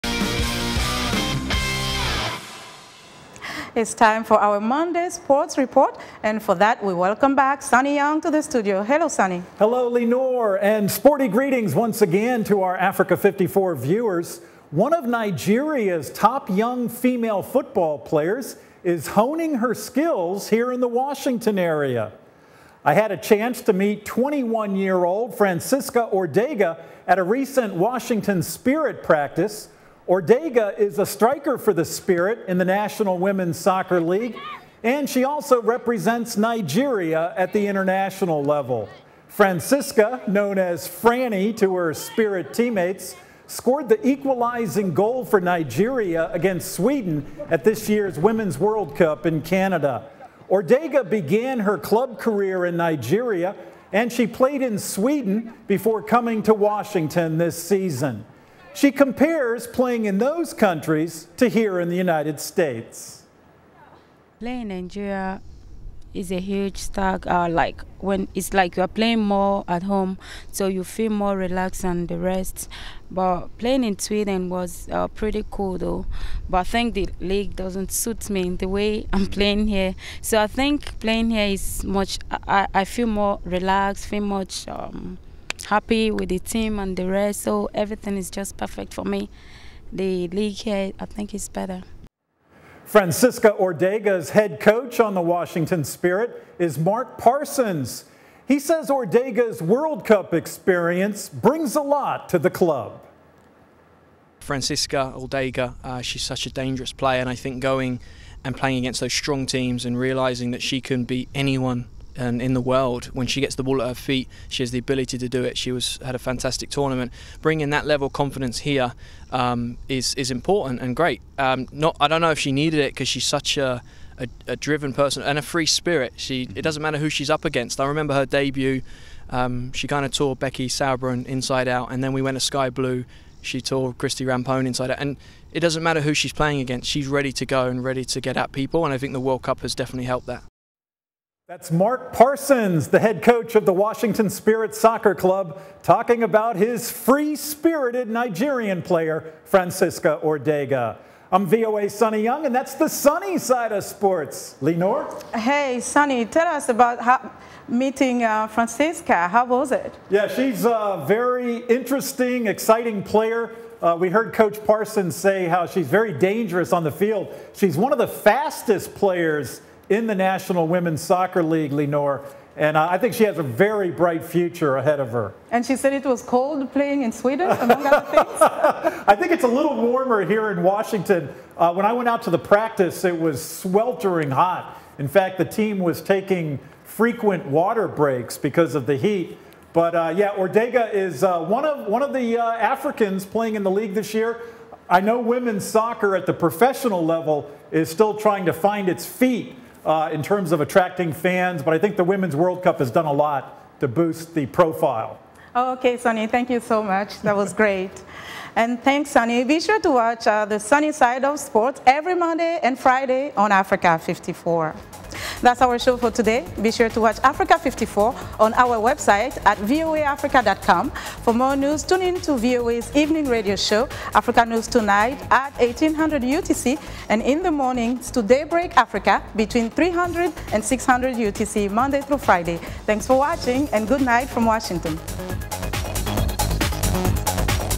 Nigeria Soccer Player Francisca Ordega
VOA’s Sonny Young, of Sonny Side of Sports, reports on one of Nigeria’s top young female football players who is honing her skills here in the Washington area. Sonny had a chance to meet 21-year-old Francisca Ordega at a recent Washington spirit practice. Ordega is a striker for the Spirit in the National Women’s Soccer League, and she also represents Nigeria at the international level.